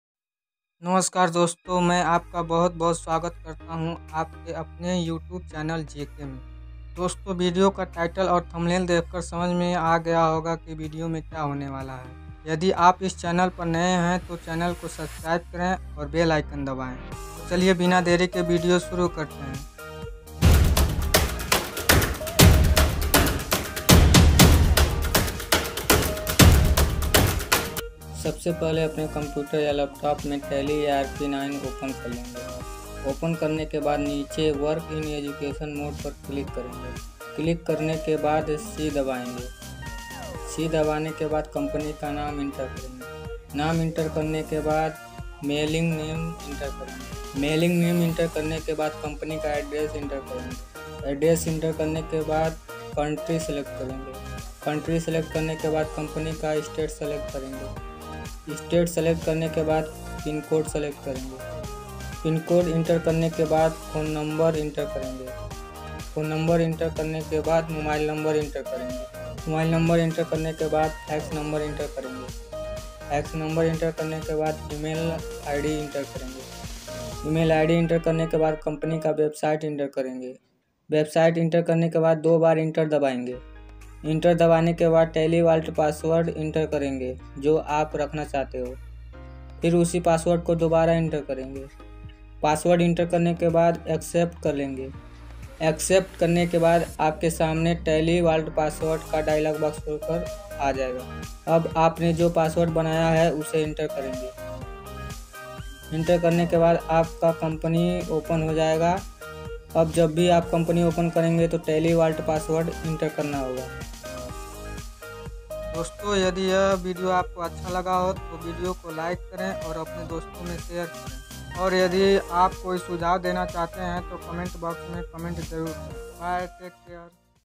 How to Make Vault Passward in Tally ERP 9 || Tally ERP 9 Me Vault Passward Kaise Lagaye
Queries Solved:-
You can easily to Create a vault password in tally erp 9 and protect your company.

JK JENERAL KNOWLEDGE